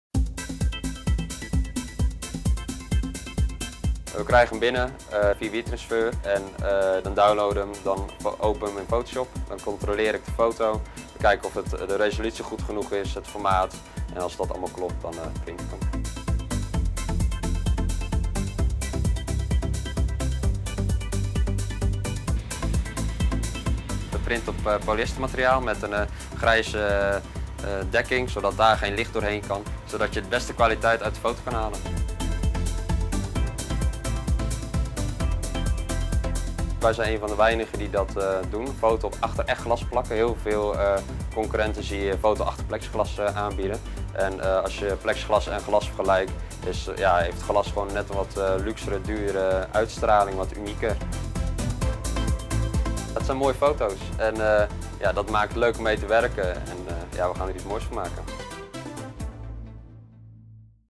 Productieproces Fine art fotografie Frank Peters door Boer en Van Wijk 1
The making off 'Venice through glass'; een tentoonstelling met fine art fotografie of fotokunst van Frank Peters In de tentoonstelling in het Dordts Patriciërshuis zijn z'n mooiste foto's van Venetië te zien. Peters werkt met lange sluitertijden waardoor beelden abstracter worden. De tentoonstelling is een onderdeel van de grote GlassFever tentoonstelling in Dordrecht. De kunstwerken worden vervaardigd door Boer en Van Wijk. Zij maken foto's achter glas voor musea, galeries of interieurs. Foto achter glas of print achter glas.